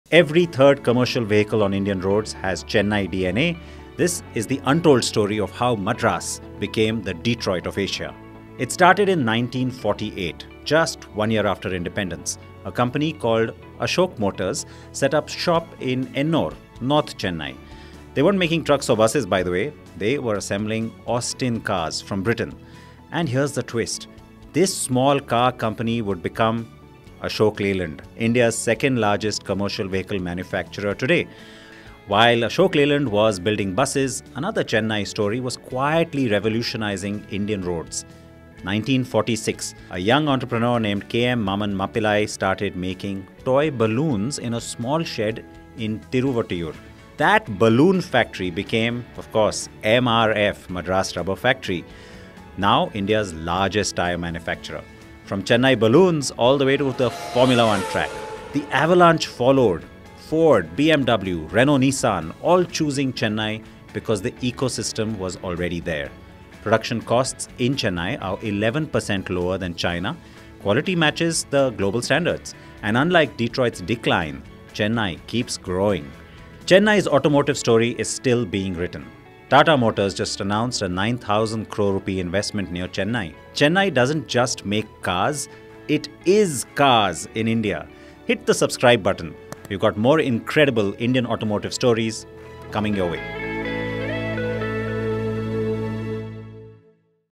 Madras Day Special: How Chennai Became the Detroit of Asia
Chennai: From Madras to the “Detroit of Asia” — A 77-Year Journey of Automotive Dominance

Discover how Chennai transformed from a small Austin car assembly plant in 1948 to India’s automotive capital—and now manufacturing a car every 20 seconds!

In this video:

The rise of Ashok Motors/Ashok Leyland, the creators of India’s first indigenous double-decker bus

The balloon-to-tire revolution that became MRF, powering everything from local roads to Formula 1

Hyundai, BMW, Ford, Renault-Nissan, and dozens more—why global automakers chose Chennai

The 60 km automotive corridor: 33% of India’s commercial vehicles, 21% of passenger cars, and 35% of auto components emerge from here .
Chennai’s edge: cost-efficiency, exports, ports, engineering talent, and EV investments like Tesla-esque tech and Tata’s ₹9,000 crore project .
Timestamped Highlights:
00:00 — 1 car every 20 seconds…
00:30 — Ashok Leyland’s origins & Titan bus
01:30 — MRF’s balloon story turned tire giant
02:30 — Hyundai & global OEM boom
03:30 — Chennai vs Detroit: cost, quality, resilience
04:00 — Madras Day link & emotional tribute
04:30 — CTA: What’s your favorite Chennai‑made ride?